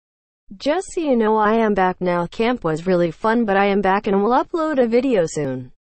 I am back now 😙✌️
I am back from camp now. It was really enjoyable and I would definitely go back there but I would miss you guys too much too stay there forever 🥺